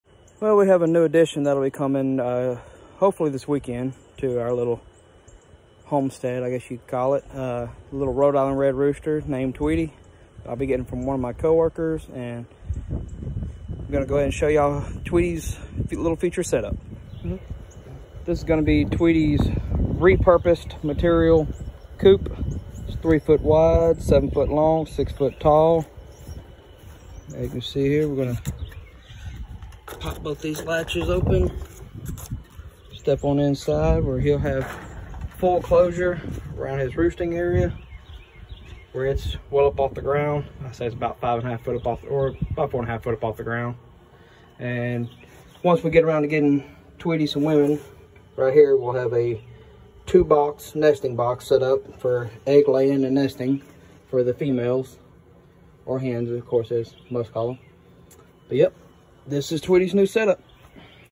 New Rooster Tweety’s future Coop made from used material
We have a new addition coming this weekend and his new setup is ready to go